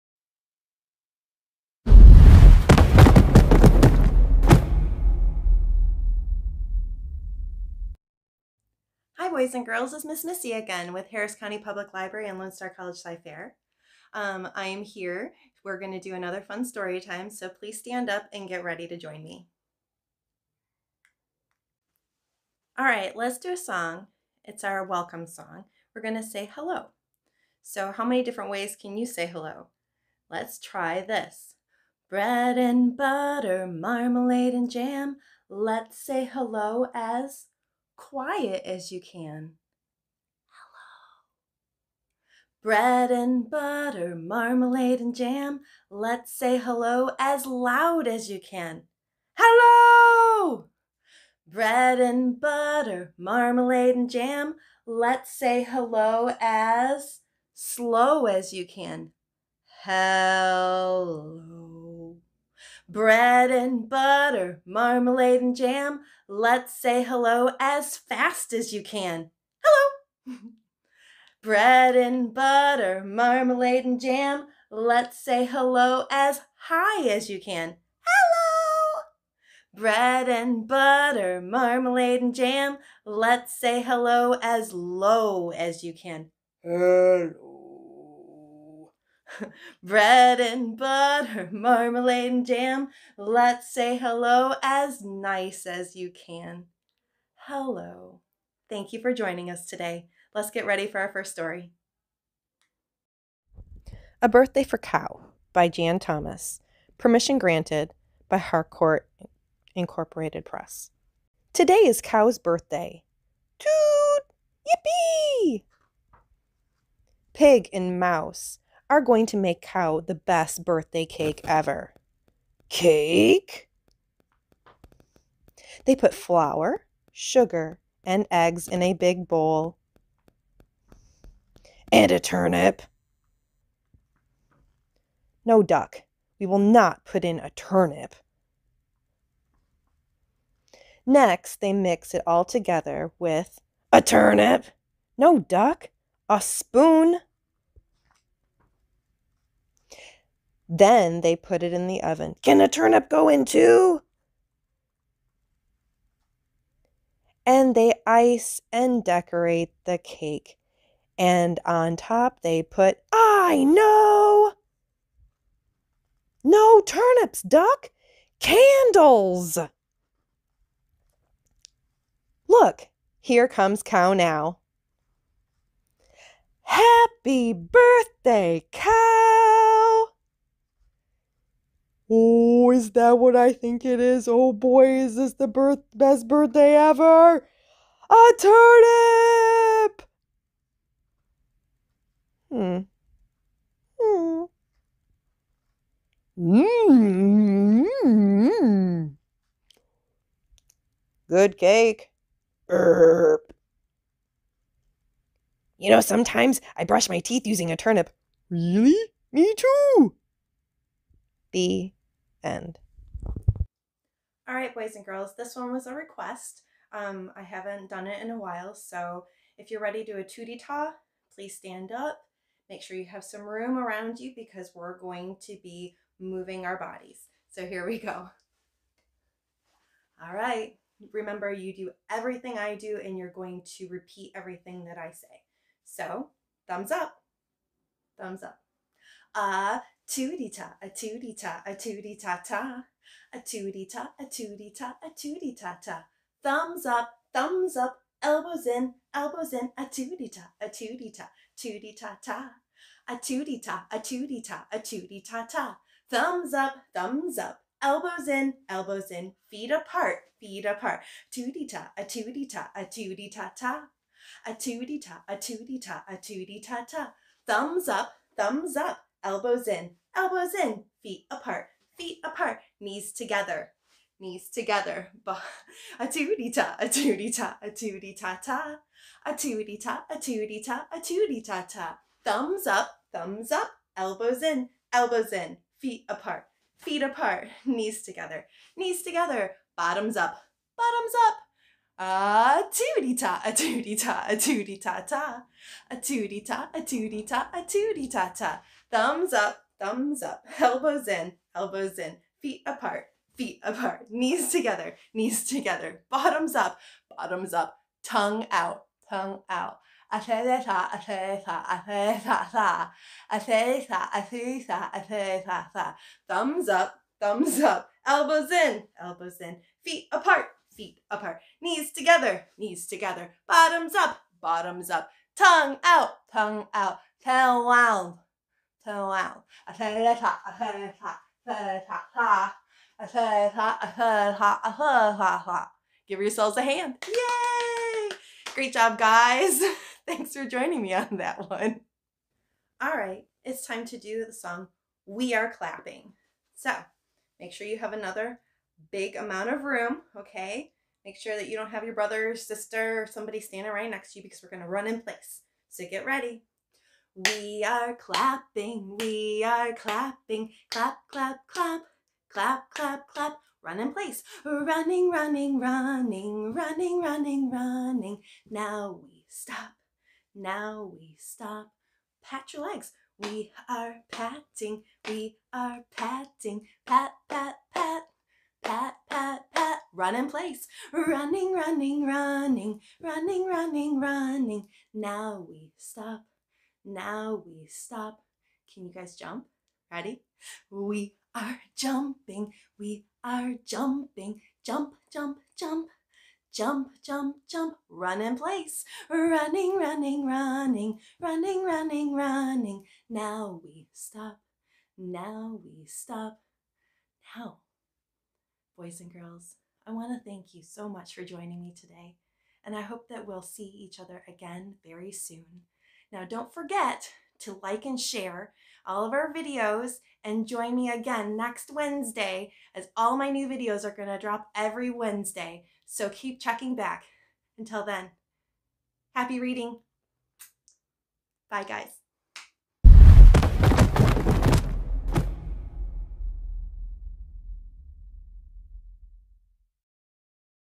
HCPL Story Time with Ms. Missy Ep 7
Join Ms. Missy as she reads "A Birthday for Cow" by Jan Thomas (Harcourt) and engages listeners into moving and grooving.